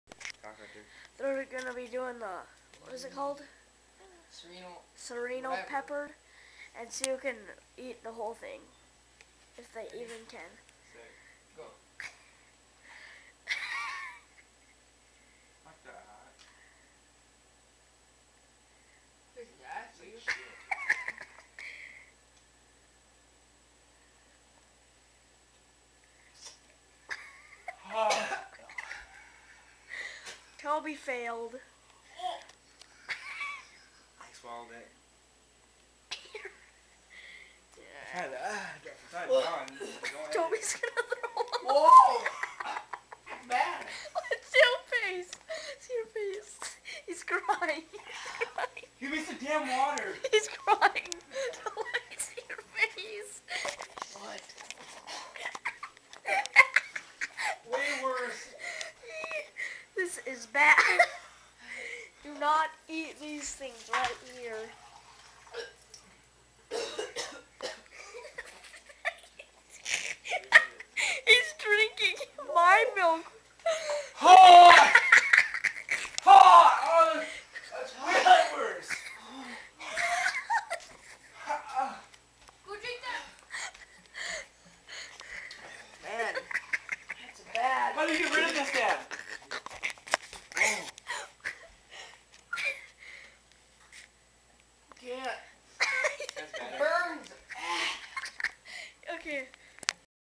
Serrano Pepper Challenge
Noah and Toby Go at the serrano pepper challenge and noah wins.